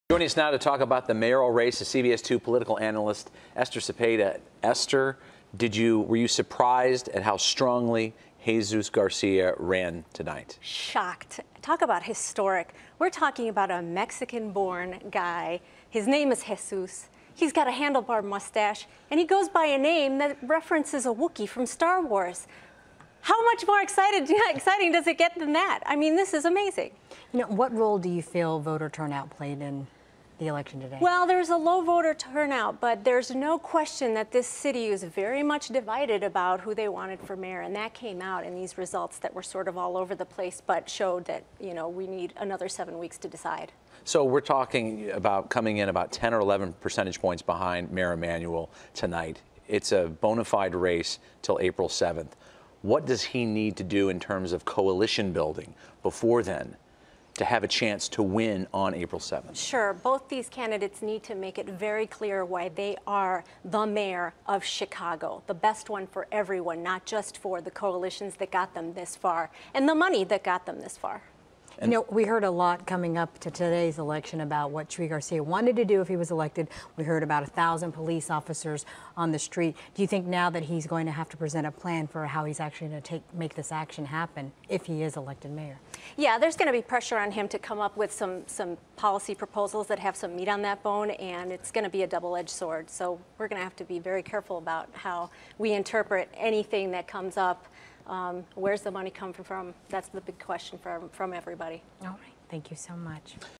Garcia Showing In Mayor's Race Surprising, Historic
CBS 2 Political Consultant Esther Cepeda discusses the meaning of Mayor Rahm Emanuel's forced runoff against Jesus "Chuy" Garcia.